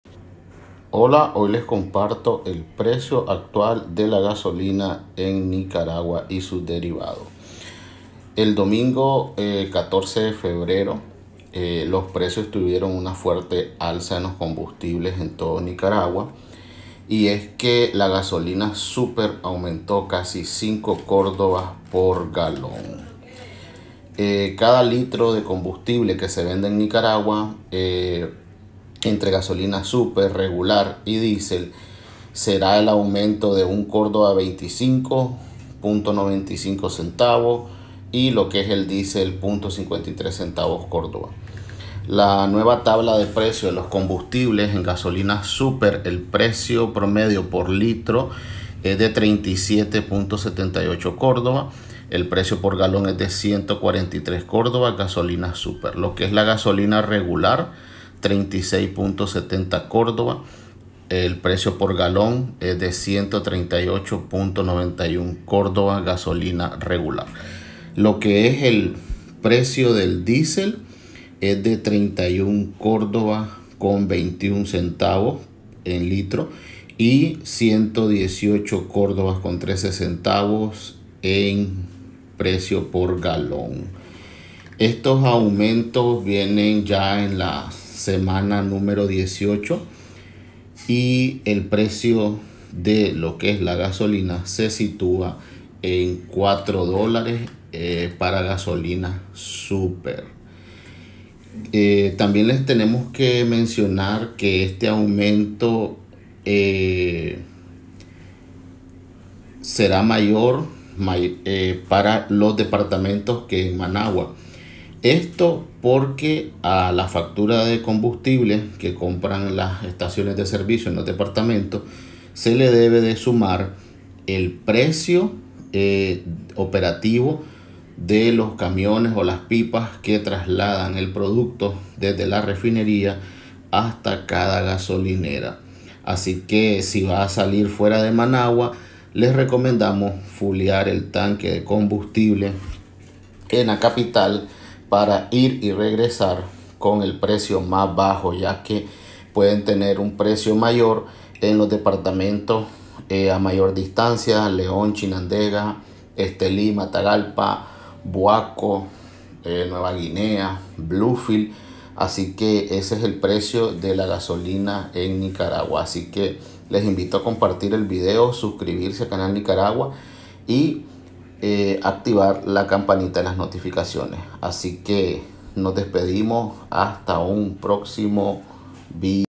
Precio de los combustibles en Nicaragua 14 Marzo 2021
Precio de gasolina en Nicaragua, aumento en sus derivados, el costo del combustible en Nicaragua para las gasolineras puma y gasolinera uno es el mismo precio en Managua, ya que en los departamentos sube por el costo operativo de transporte, así que esto es una idea de cuanto cuesta vivir en Nicaragua, como es Nicaragua, como es managua con el precio de la gasolina super en las estaciones de servicios en el pais, estos precios son para gasolineras en managua nicaragua.